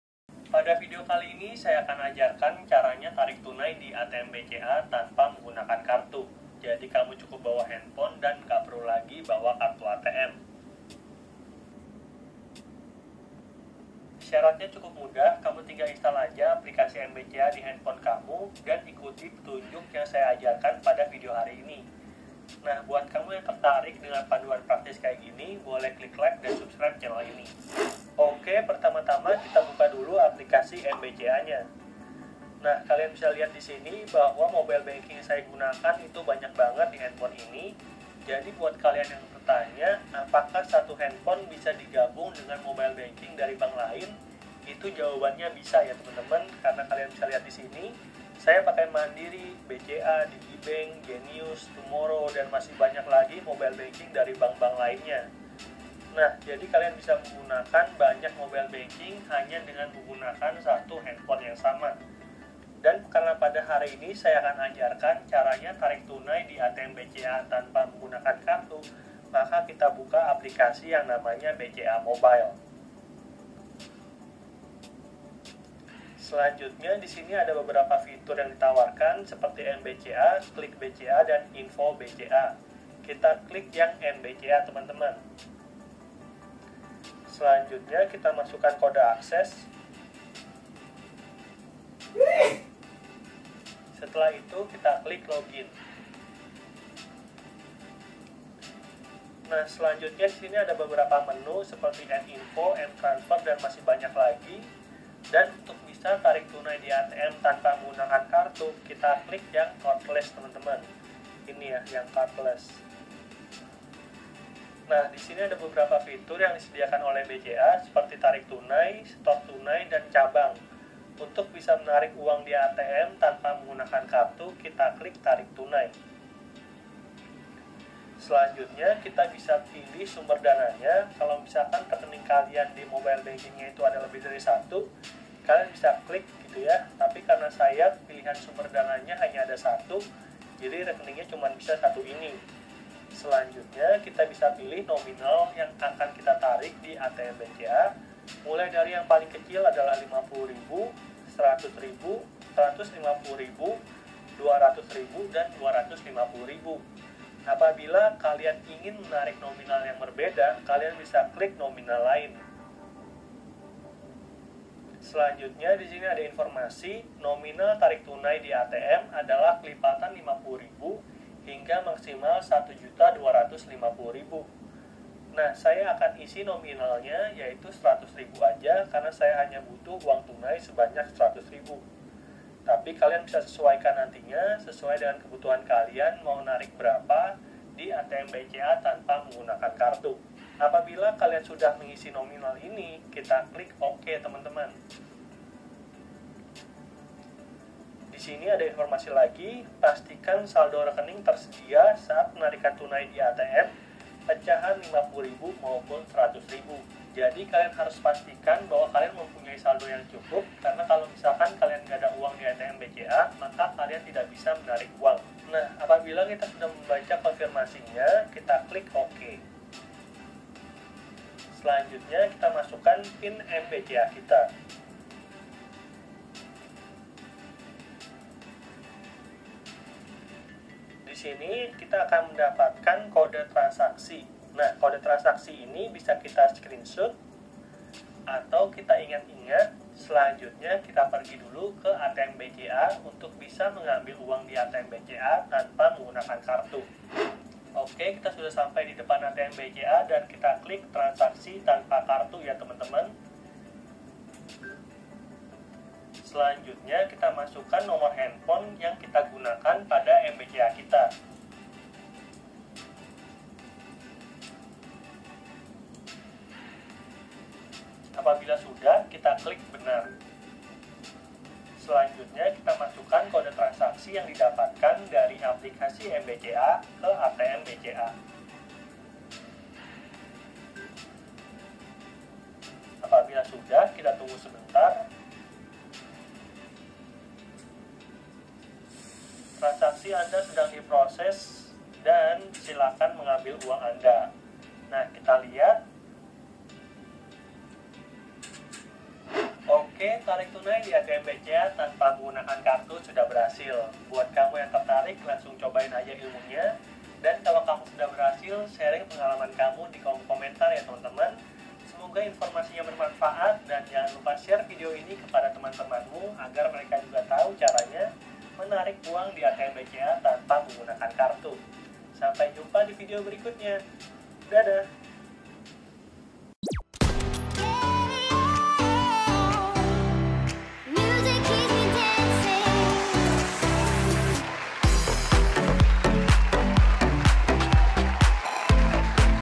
CARA TARIK TUNAI DI ATM BCA TANPA KARTU DEBIT // BCA MOBILE
Suaranya ga terdengar kalau pakai speaker HP. Jadi harus pakai headset. masalah ajaib yg sampai sekarang belum ketemu solusinya. harap maklum ya.

Berkat kecanggihan aplikasi BCA Mobile, kita dapat mengambil uang di ATM BCA tanpa menggunakan kartu. BCA Mobile menerapkan sistem kerja dengan menggunakan kode transaksi. Kode transaksi ini bersifat rahasia dan hanya berlaku selama satu jam setelah mendapatkan kode tersebut dari BCA Mobile.

Jadi kamu yang mengalami kendala di kartu ATM seperti : 1) ATM yang rusak sehingga tidak terbaca mesin, 2) kartu ATM tertelan dalam mesin, bahkan 3) ketinggalan kartu ATM ... tidak perlu bingung dan repot lagi! Cukup install aja aplikasi BCA Mobile di HP kamu dan seluruh permasalahan tersebut akan teratasi.

Saya akan mengajarkan cara tarik tunai di ATM BCA tanpa kartu debet di video ini. Semoga bermanfaat & selamat menonton. ENJOY!